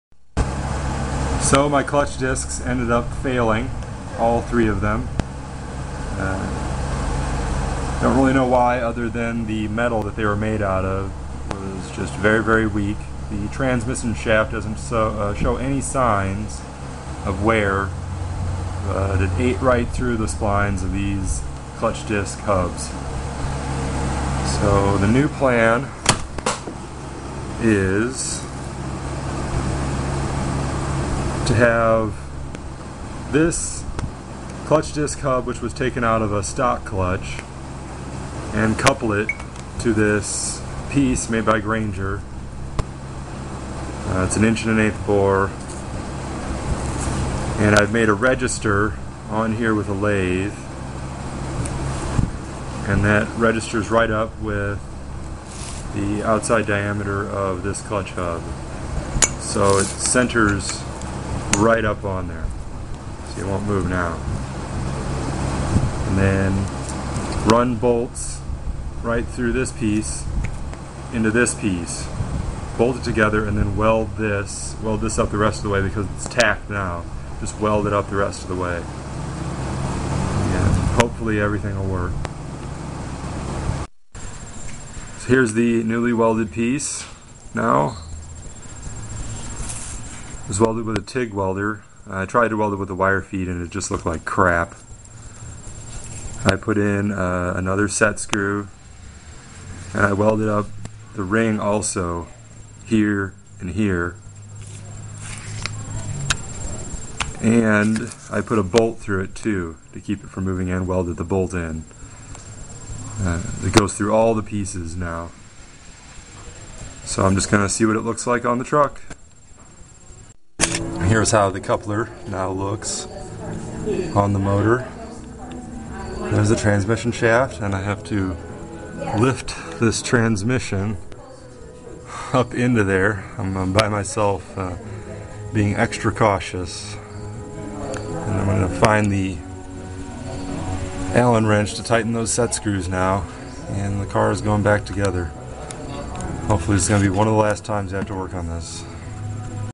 Electric B2200: New Motor Coupler
My Three Disc Tilton Motor Clutch failed so I guess its back to having the motor shaft directly coupled to the transmission shaft only this time it will be lighter and more balance because there won't be as much weight hanging off the shaft.